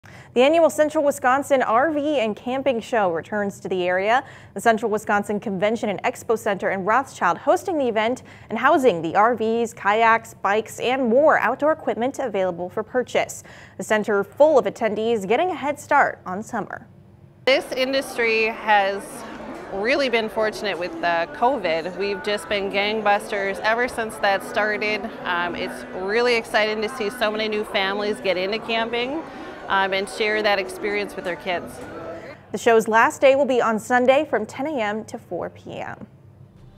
Annual Central Wisconsin RV and Camping Show returns to Rothschild
The annual Central Wisconsin RV and Camping Show returned to Rothschild over the weekend.